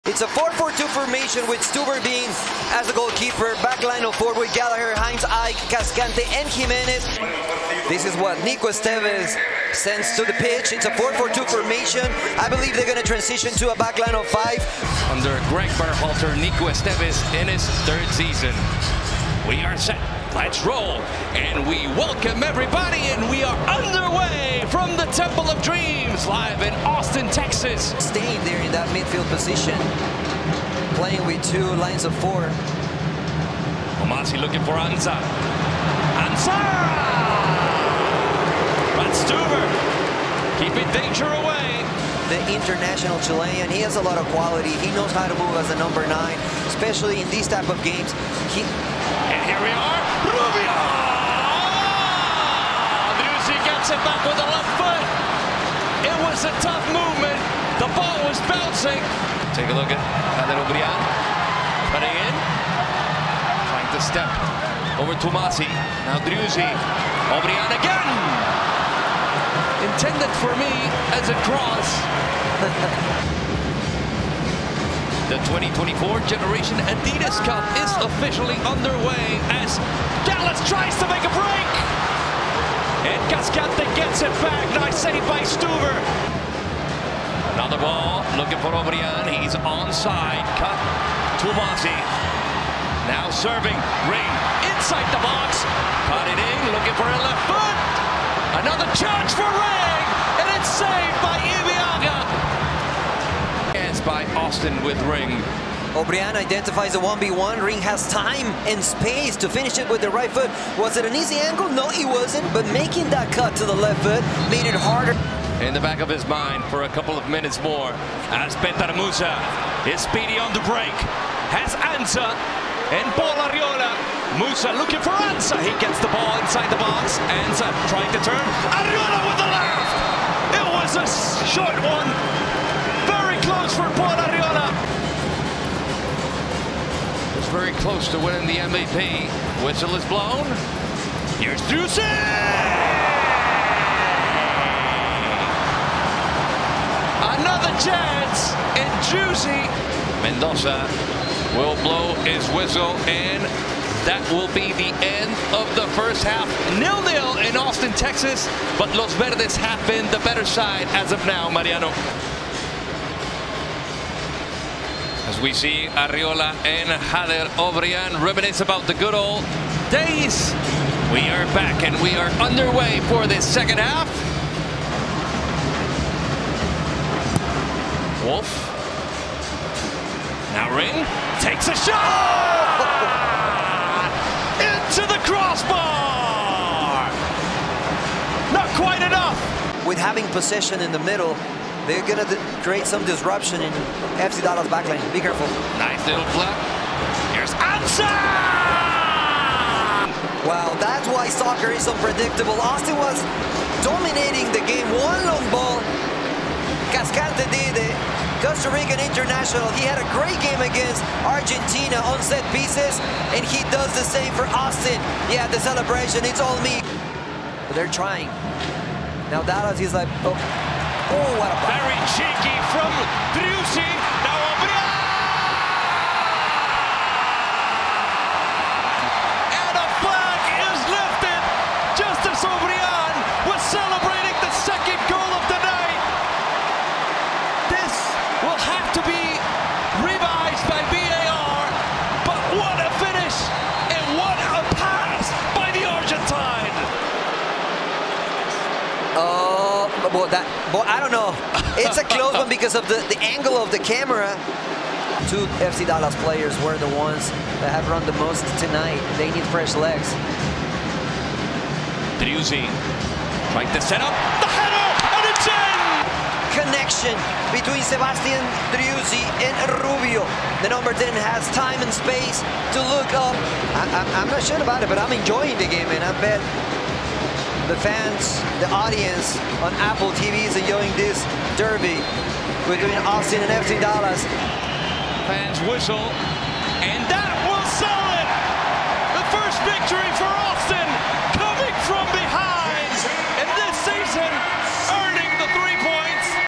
FC Dallas at Austin FC Highlights | March 30, 2024
FC Dallas falls 2-1 to Austin FC on March 30, 2024.

0:00-0:28 Kickoff
0:29- 2:42 First Half Action
2:43-3:09 Second Half Action
3:10-3:26 Eugene Ansah Goal for FC Dallas
3:27-3:36 Goal for Austin FC
3:37-4:15 Offside
4:16-4:33 Goal for Austin FC
4:34-4:56 End of Second Half